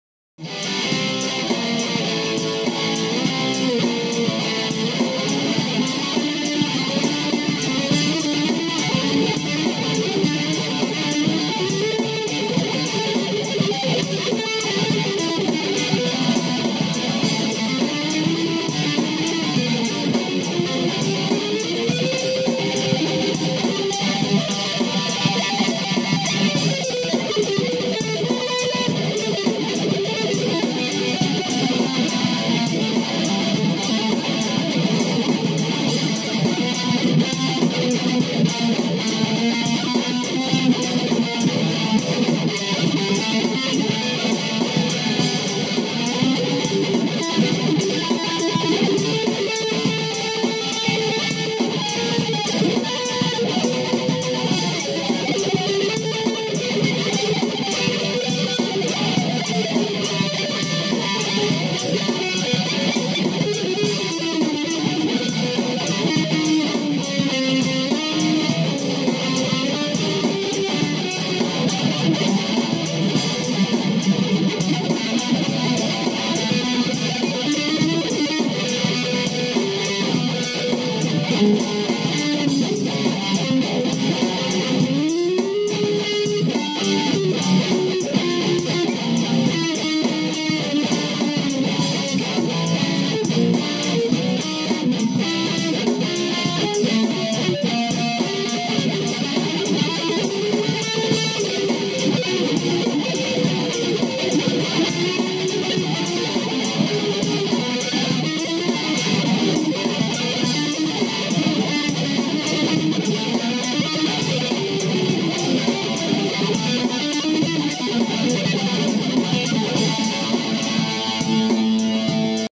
BRANO HEAVY METAL
BRANO HEAVY METAL COMPOSTO DAL CHITARRISTA HEAVY METAL FAMOSO NEL MONDO DOMENICO BINI